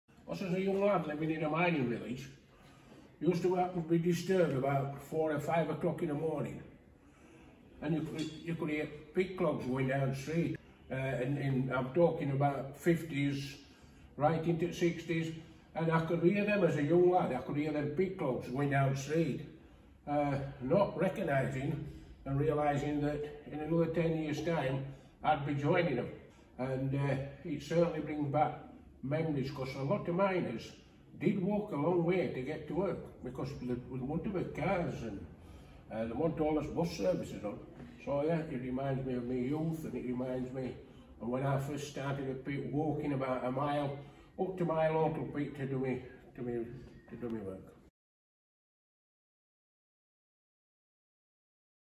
Drawn from the Pits: Eric's Favourite Painting
One of the most exciting things about reopening has been welcoming visitors in to see our brand new art exhibition, Drawn from the Pits.

The exhibition showcases the artwork of several miners-turned-artists, and today we're catching up with Eric to discover what this painting, Pit Village by Tom McGuinness, means to him.

If you like this and want to hear more from our miners, you can purchase one of our new guide books when you visit us!